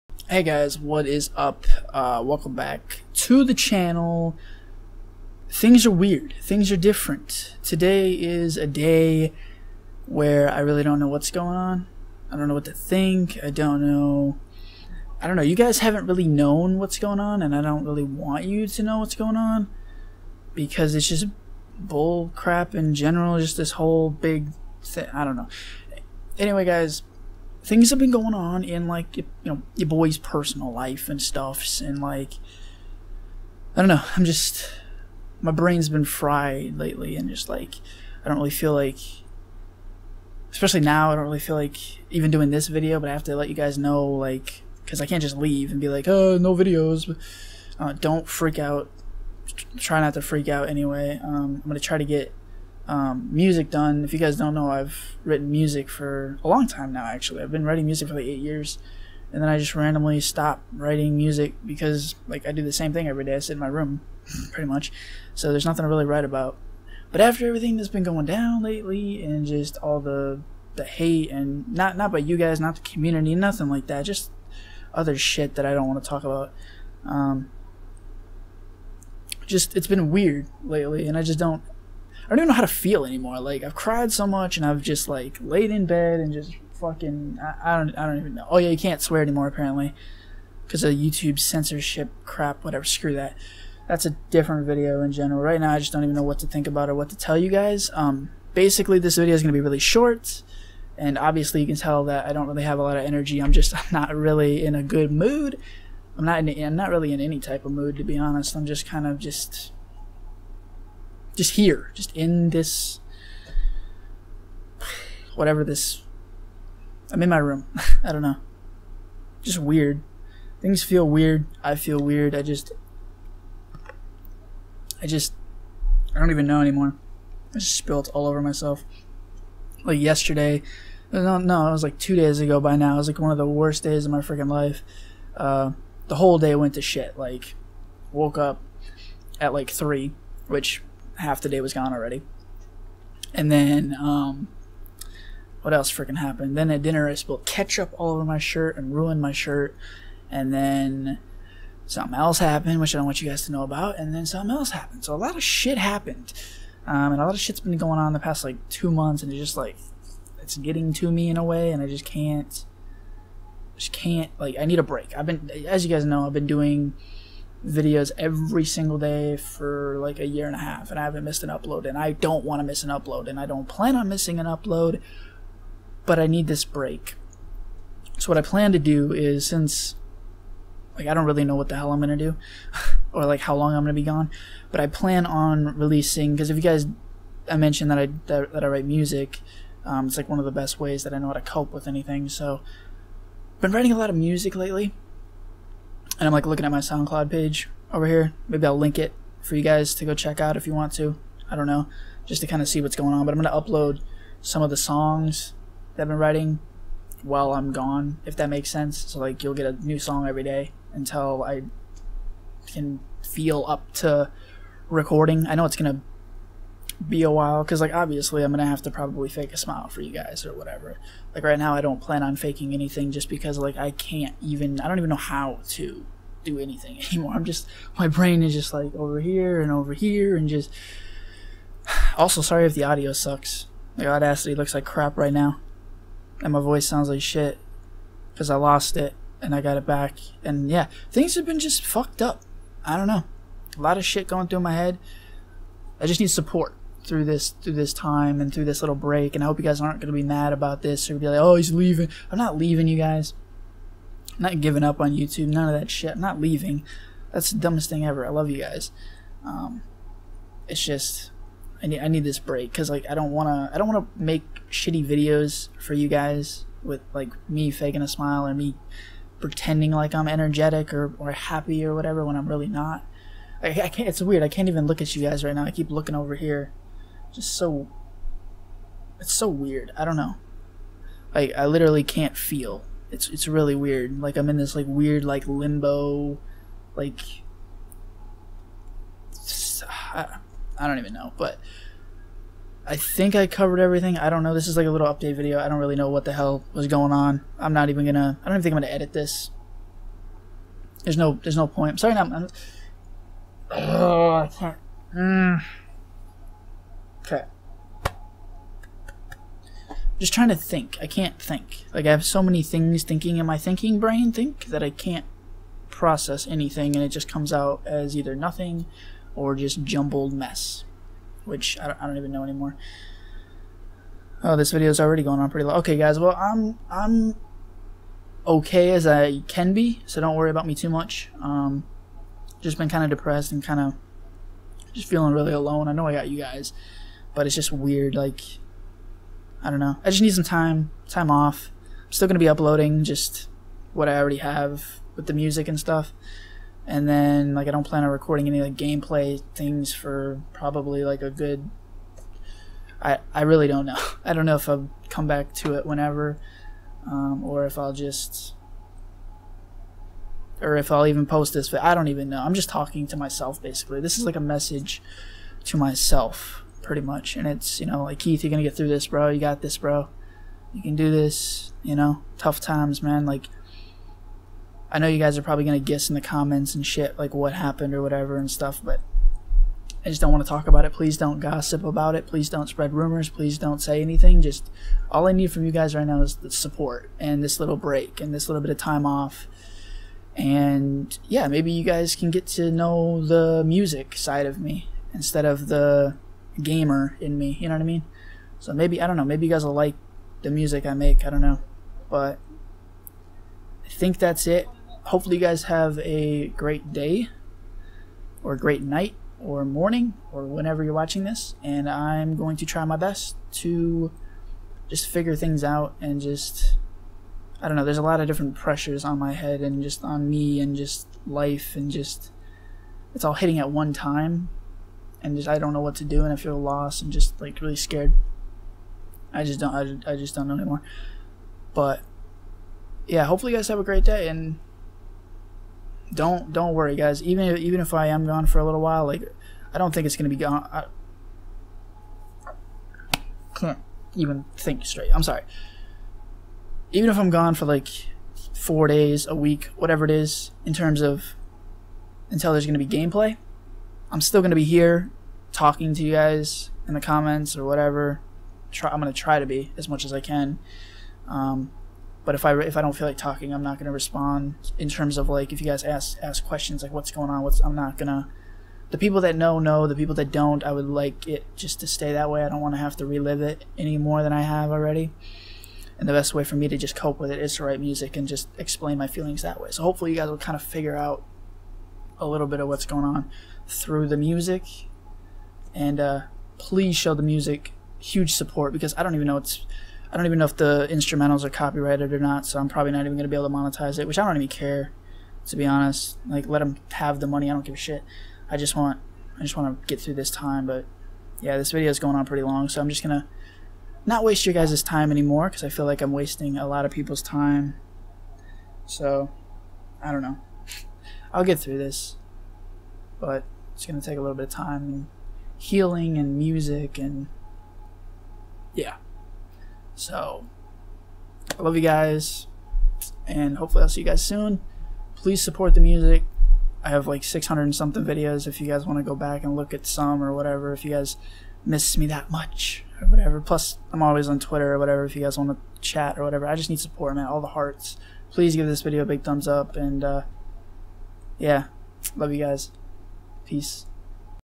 taking a break.
I need a small break. things are going every which way in my life right now and i don't have the energy to crank out a video every day at the moment. please don't be disappointed. i need all the support right now.. I love you guys

Public Skype! ►Kabooshkies